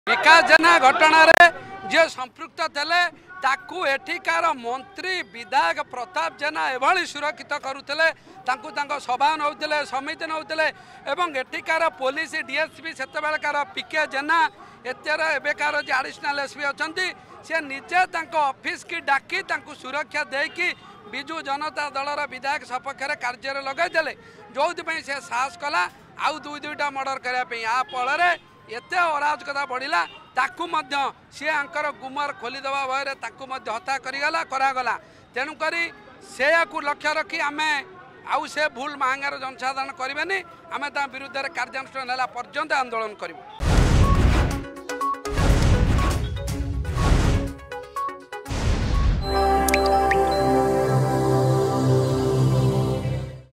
ବିଜେପି ପଦଯାତ୍ରା...
ଯେ ପର୍ଯ୍ୟନ୍ତ କାର୍ଯ୍ୟାନୁଷ୍ଠାନ ନନିଆଯାଇଛି ଆନ୍ଦୋଳନ ଜାରି ରହିବ - ବିଜେପି ନେତା ଶାରଦା ନାୟକ...